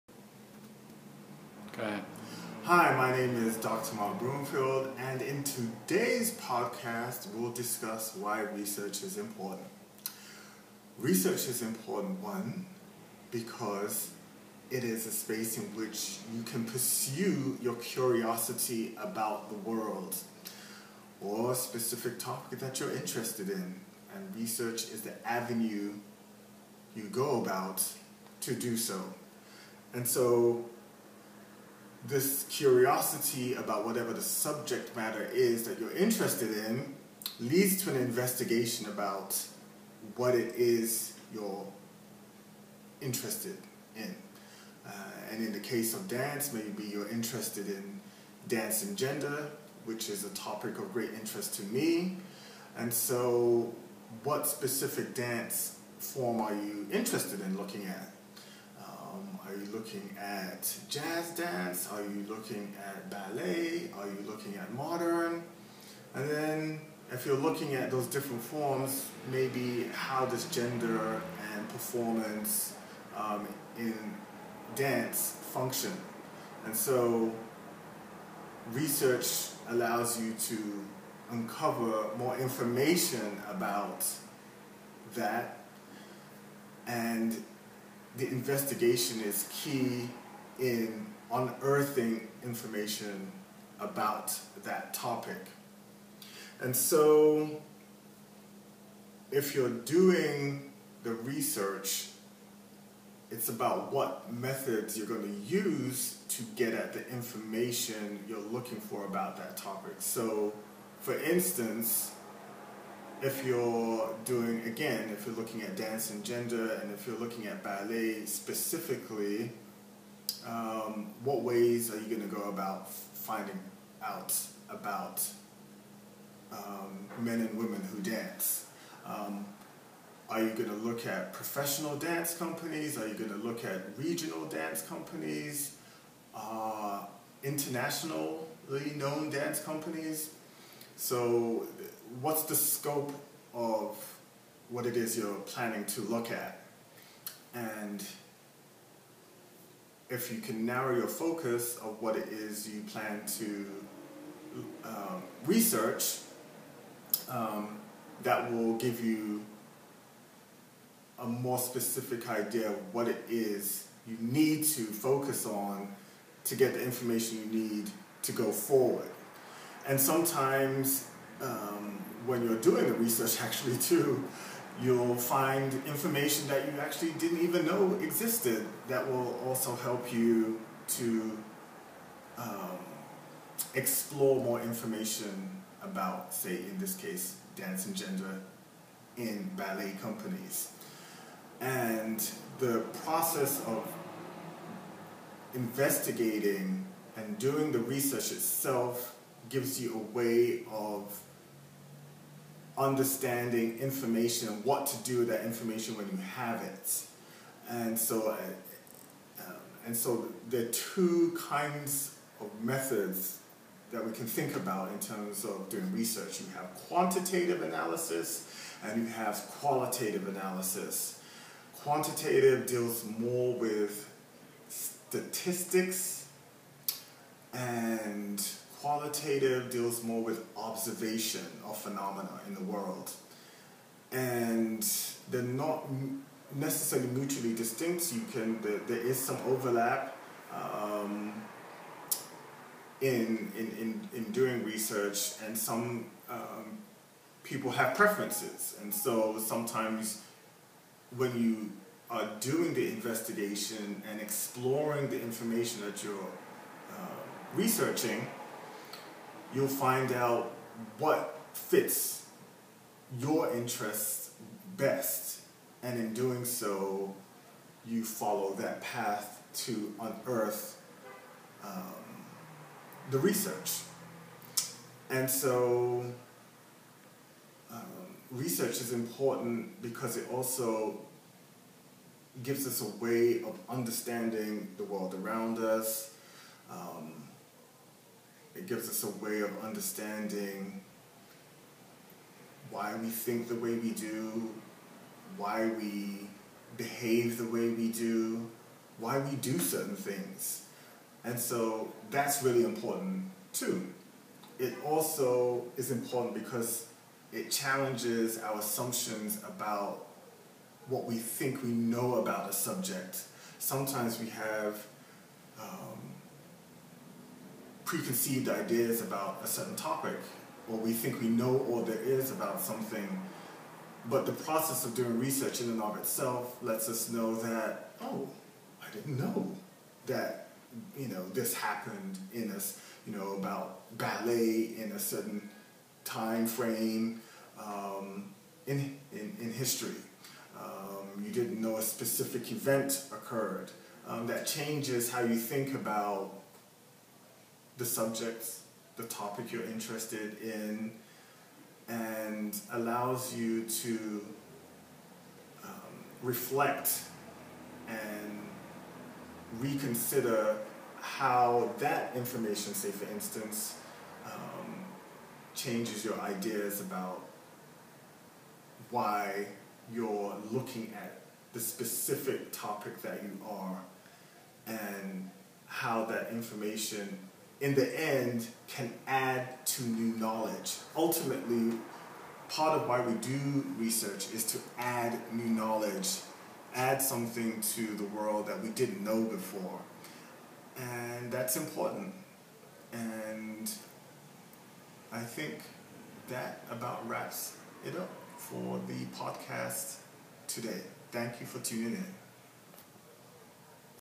Why Is Research Important?
Mark Broomfield, PhD, discusses the importance of research.